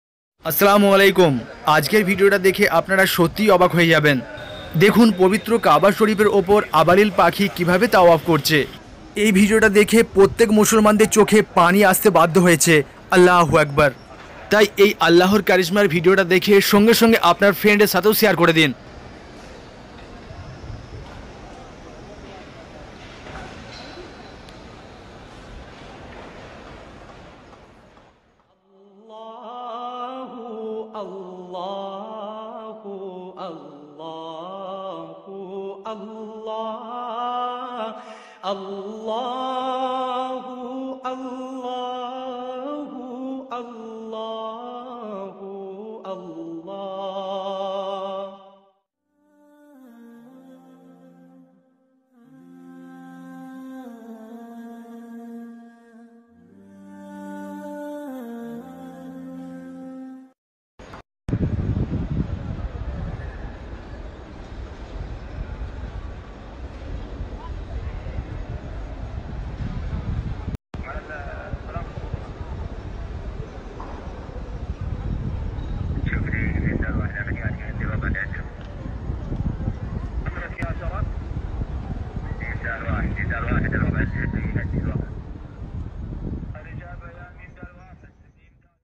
পবিত্র কাবার ওপর আবাবিল পাখি/ করনা আতঙ্ক দুর করতে আল্লাহ করিশমা দেখুন
পবিত্র কাবার ওপর আবাবিল পাখি আল্লাহ করিশমা দেখুন ।
ভিডিওটা আপনার সমস্ত বন্ধু দের কে পাঠিয়ে দিন তাঁরাও যেন এই ভিডিওটা দেখতে পারেন।